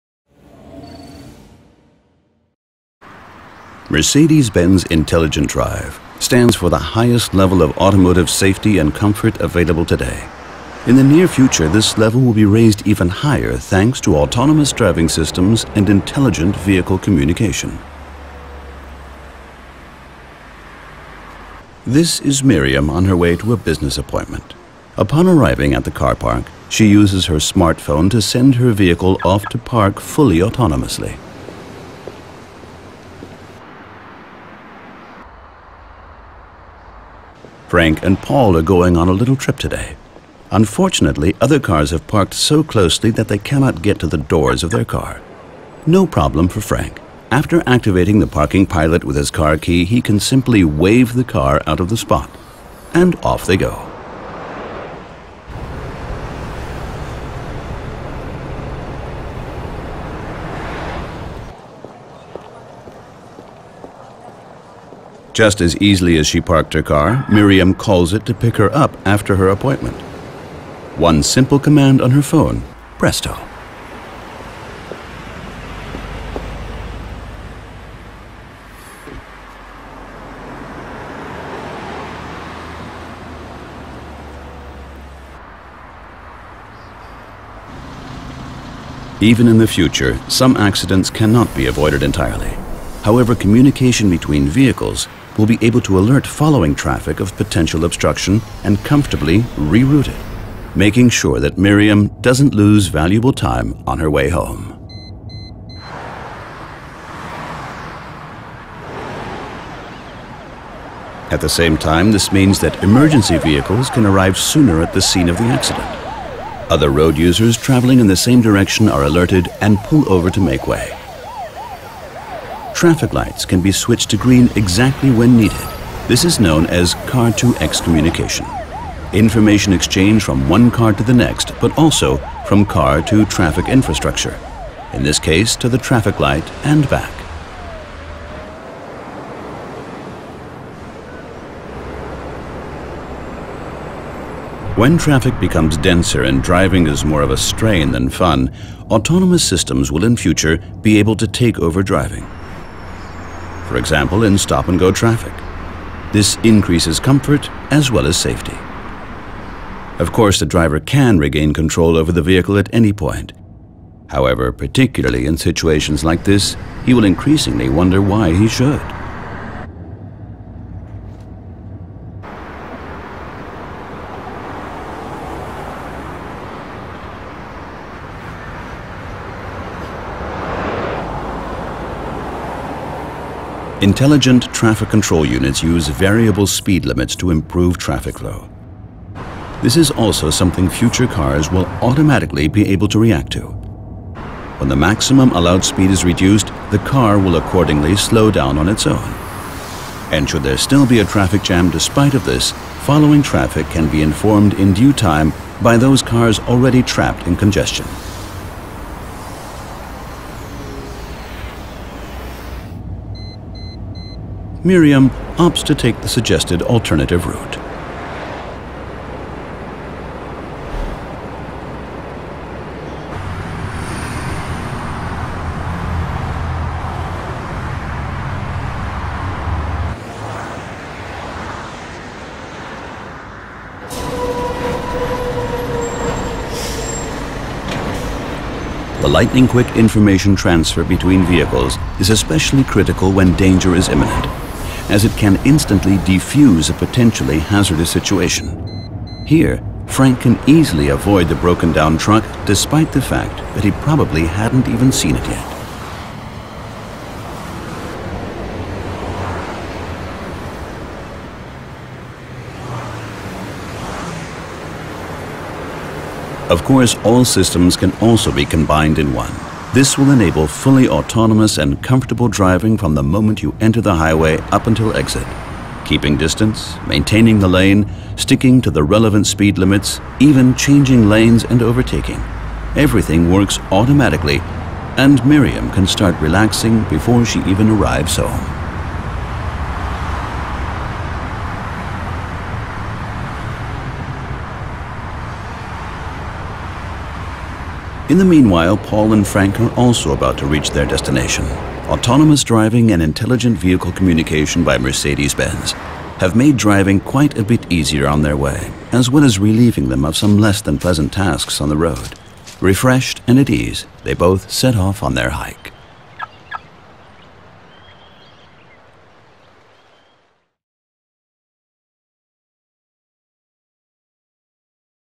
Mercedes-Benz Intelligent Drive
Mercedes- Benz Intelligent Drive staat voor het hoogste niveau van veiligheid en comfort in de auto. In de nabije toekomst wordt dit niveau verder verhoogd, dankzij autonoom rijden en intelligente communicatie tussen voertuigen onderling.